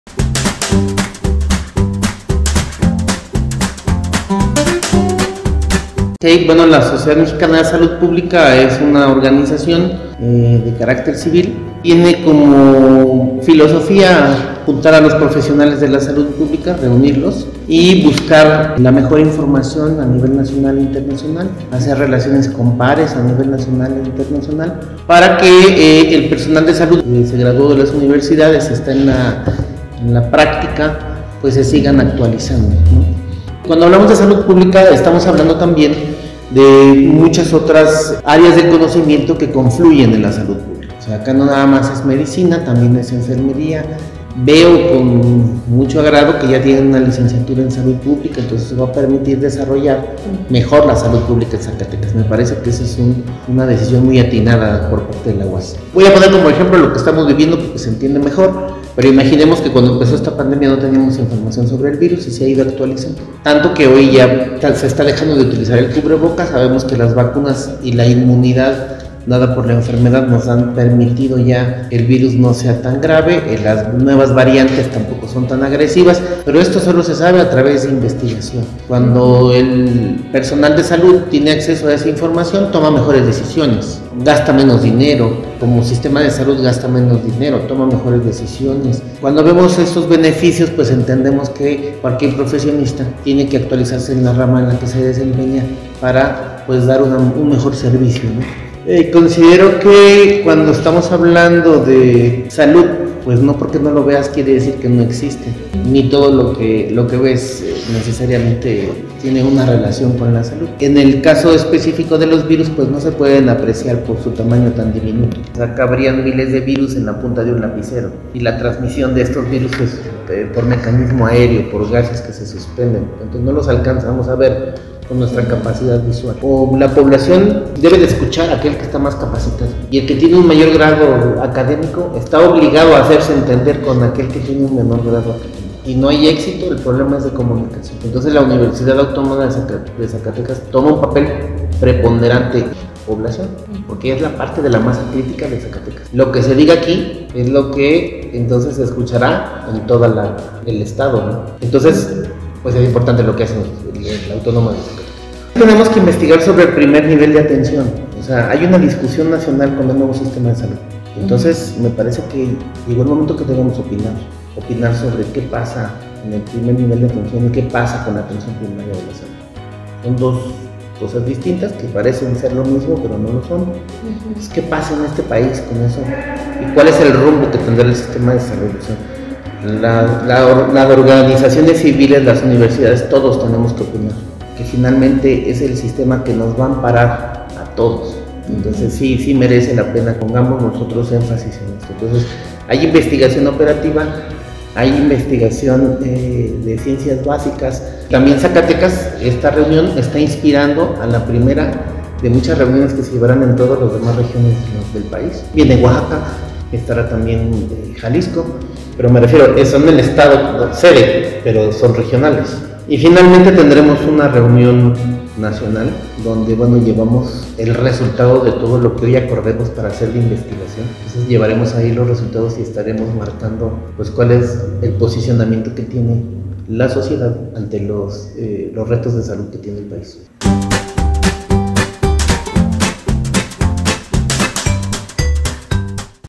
La importancia de la Salud Pública en Zacatecas
El Dr. Juan Francisco Martínez Campos, presidente de la Sociedad Mexicana de Salud Pública, habla sobre los retos que existen en el tema de salud pública a nivel nacional y la importancia de poder realizar investigación en este tema.

Así mismo, mencionó la importancia que tiene Zacatecas en el tema de Salud pública, pues es una de las pocas universidades a nivel nacional que cuentan con esta carrera, dando oportunidad a la verdadera profesionalización de los estudiantes.

Lo anterior es también una área de oportunidad pues pocos son los profesionales en salud publica que cuentan con la formación profesional desde las bases, es decir, desde su formación universitaria.